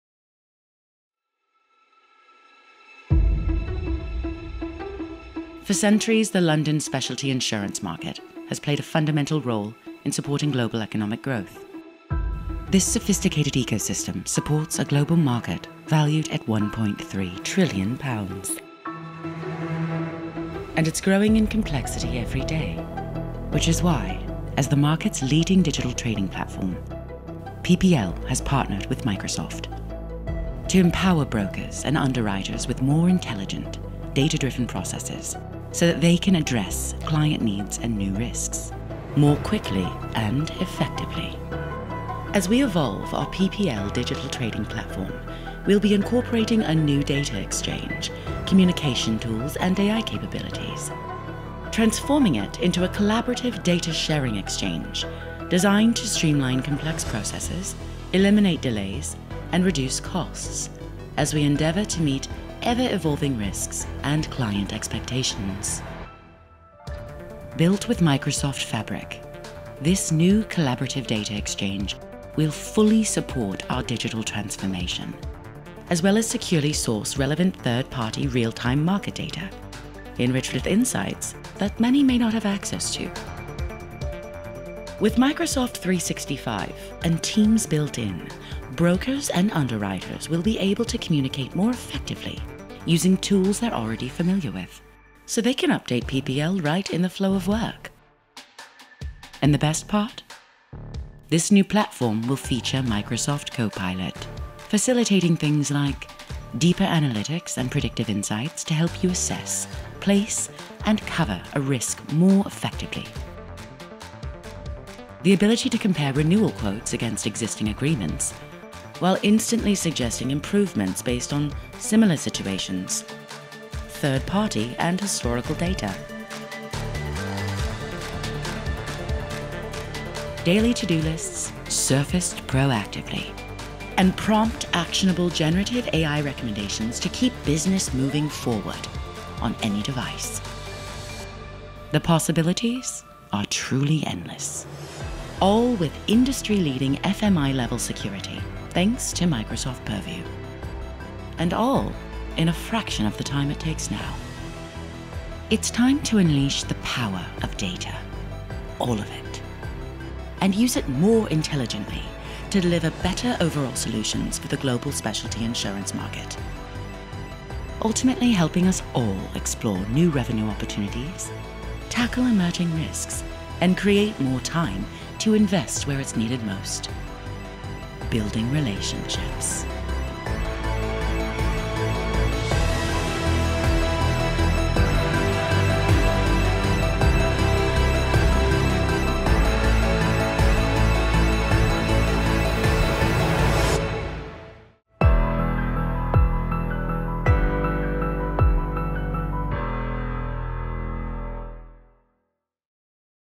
PPL builds new trading hub using Microsoft tech
Placing Platform Limited (PPL) has announced a multi-year agreement that will integrate Microsoft’s data, AI and productivity tools into the flow of work on PPL’s speciality insurance trading platform.